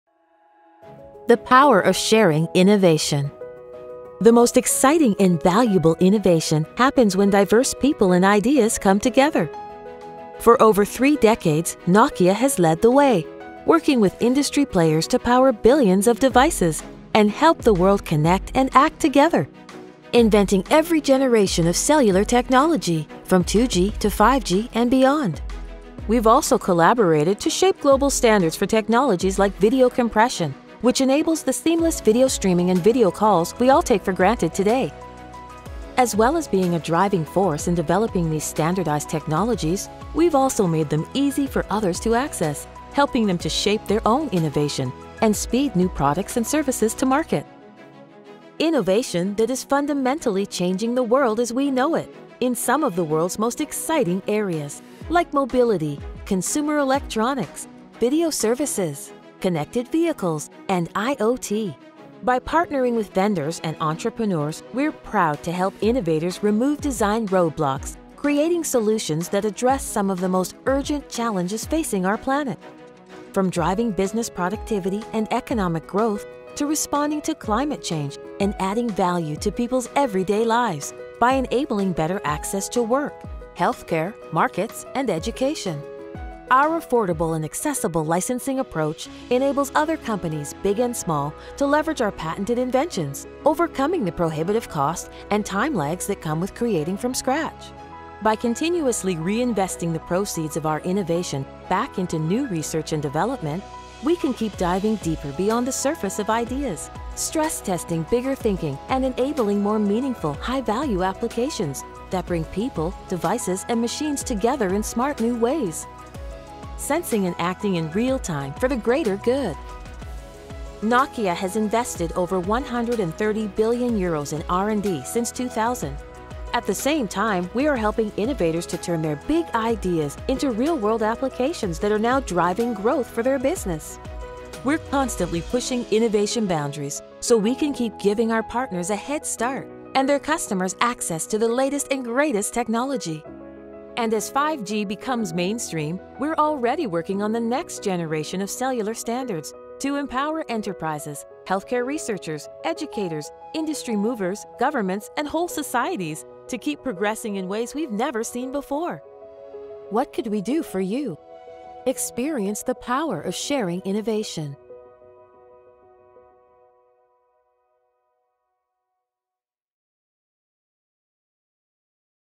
Nokia licensing: The power of sharing innovation
Nokia patent licensing unleashes the power of shared innovation, bringing together diverse ideas to drive technologies from 2G through 5G and beyond.

For more than 30 years, we’ve co-created global standards in areas like cellular communication and video compression, and shared our inventions through licensing.